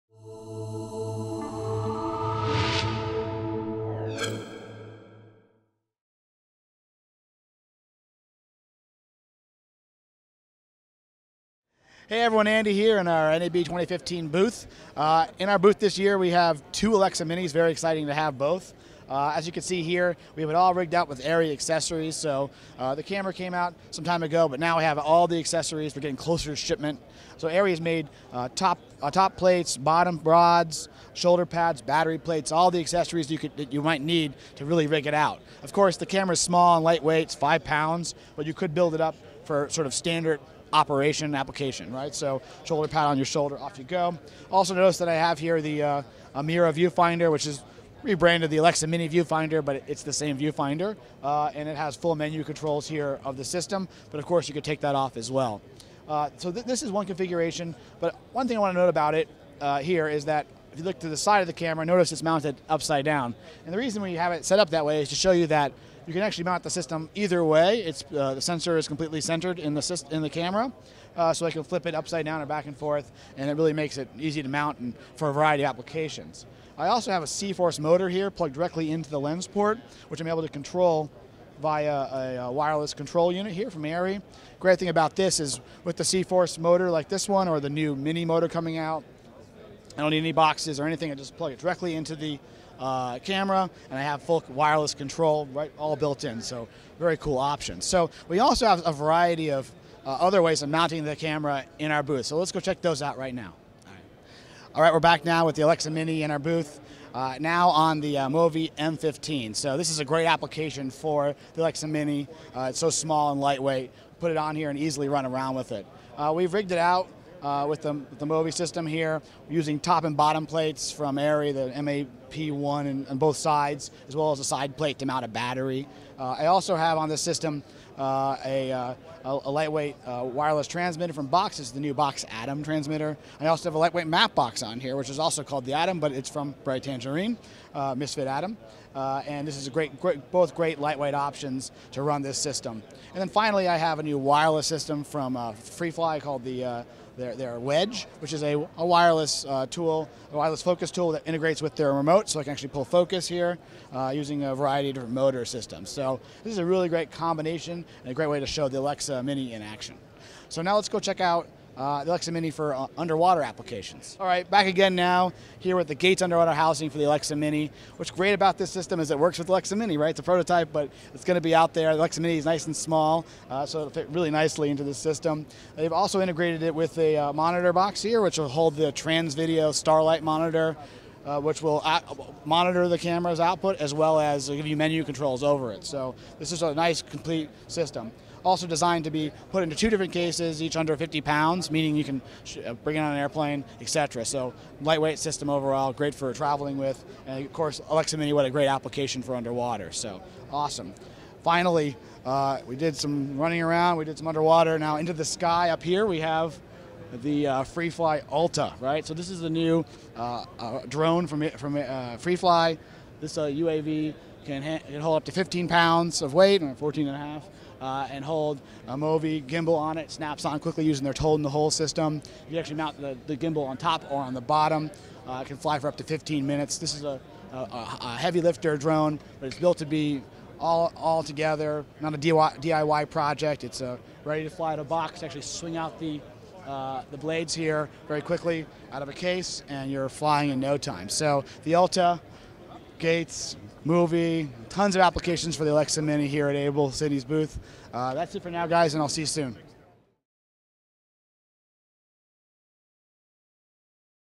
NAB 2015: Camera Mobility with the ALEXA Mini
We recently covered the breakthrough mobility of the ALEXA Mini and we have the camera in our NAB booth this year. In the first configuration, it is fully rigged out with Arri accessories including a shoulder plate and battery plate. Although the camera is small and lightweight at 5 lbs, it can be built up for standard operation. You'll also notice that the ALEXA Mini is actually mounted upside down. You can mount the camera either way as the sensor is completely centered, making it usable for a variety of applications.

Second, we mounted the ALEXA Mini on the MōVI M15 and rigged it out using lightweight options including top and bottom plates from Arri and the Misfit Atom Mattebox from Bright Tangerine. I also used the new wireless system from Freefly, the WEDGE, which is a wireless focus tool that integrates with the remote to pull focus using a variety of different motor systems.

Finally, I looked at underwater and aerial applications. Gates recently announced their underwater housing for the ALEXA Mini and we have a prototype in our booth. Not only is the housing designed specifically for the ALEXA mini, it includes a monitor box that will hold the transvideo starlight monitor that monitors the camera's output as well as provide menu controls. Lastly, the Freefly ALTA, the latest drone from Freefly, is a great aerial application for the ALEXA Mini. It can hold up to 15 lbs, and a MōVI gimbal can snap on easily using their Toad in the Hole system. The gimbal can be mounted on the top or bottom and the ALTA can fly for up to 15 minutes.